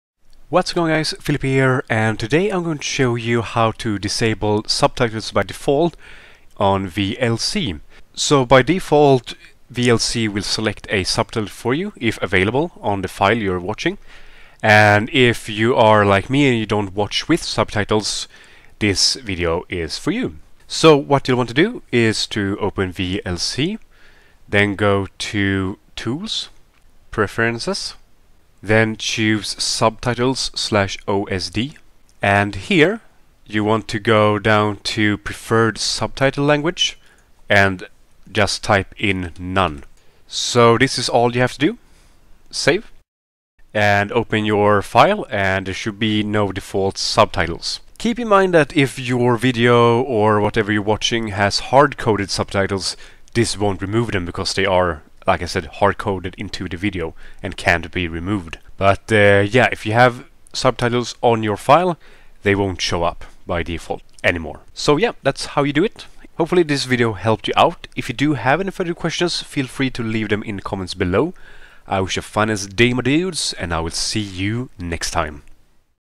VLC - CHANGE DEFAULT SUBTITLE TO NONE (DISABLE DEFAULT SUBTITLES) [2025]
Here is a video on how to remove the default subtitle for VLC media player.